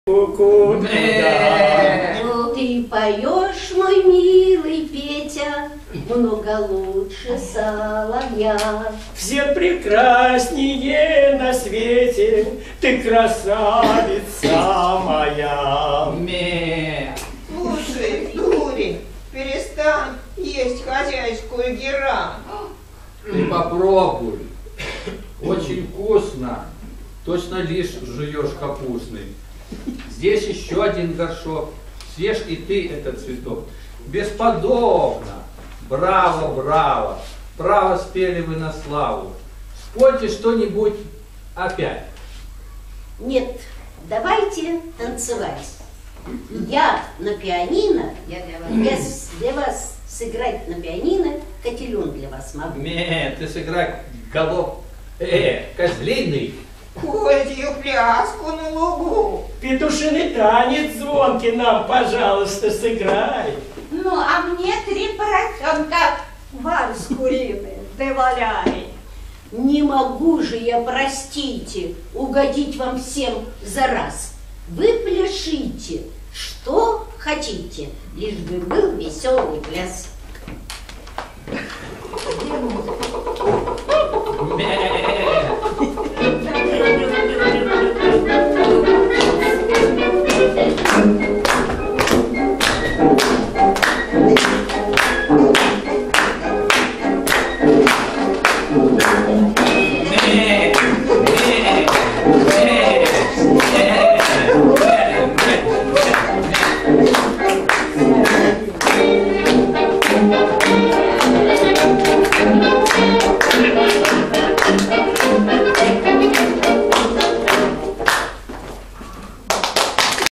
Копия видео "Кошкин дом. Спектакль для детей"
В День защиты детей читатели библиотеки приготовили для ребят сюрприз - спектакль "Кошкин дом"